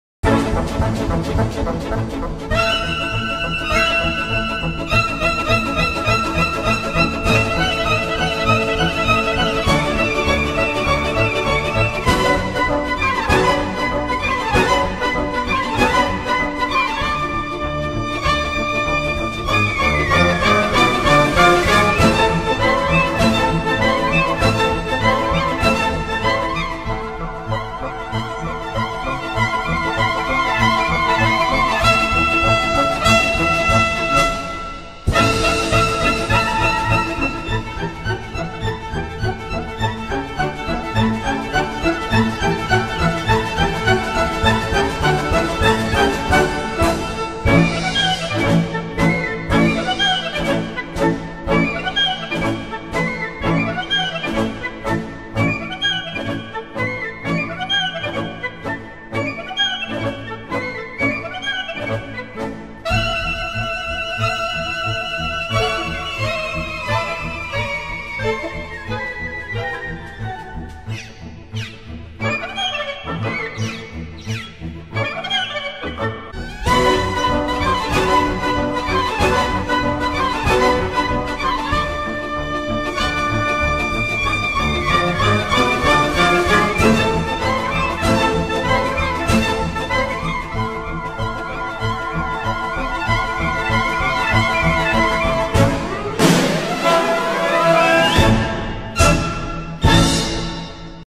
Russia State academic folk dance Russians are awesome we love Russia
رقصة رائعة من الفلكلور الروسي